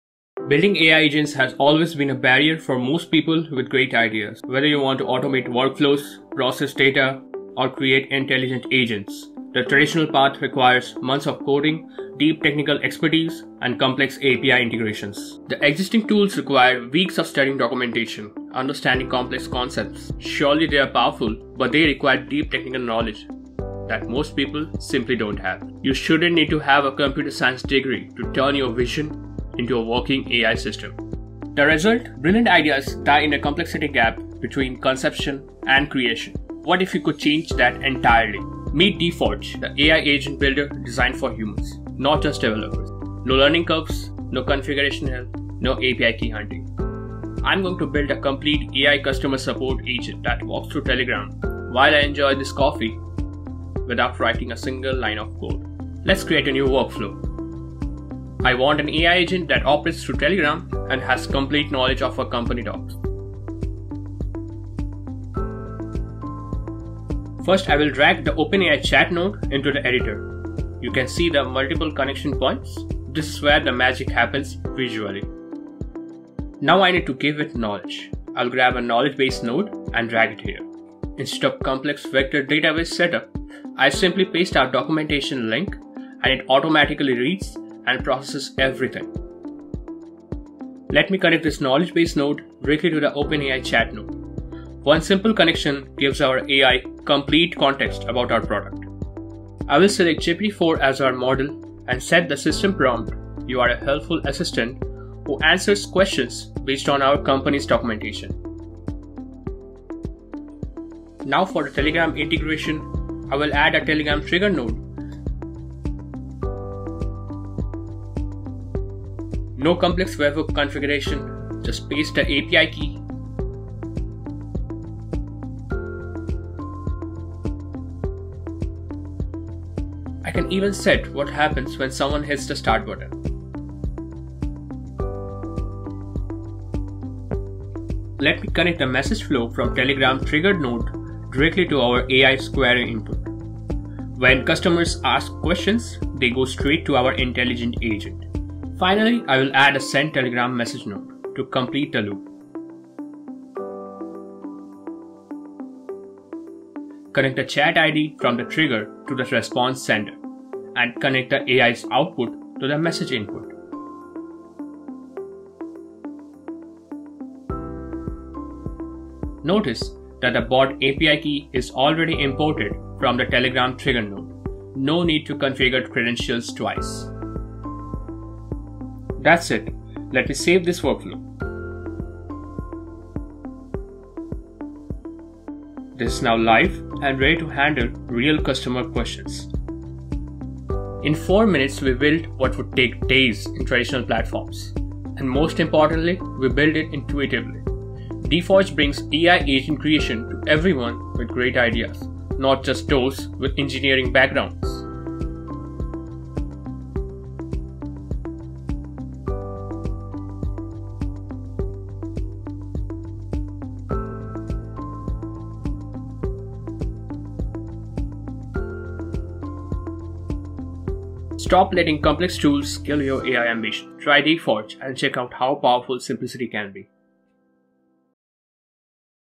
Introducing Deforge - A Node Based AI Agent Builder
For months, this is what we've been building.
Countless late nights, endless feedback loops, and a relentless focus on making AI accessible to everyone. I'm incredibly proud of what the team has built.
If you've ever wanted to build a powerful AI agent but were blocked by code, this is for you. Join our closed beta and let's build together.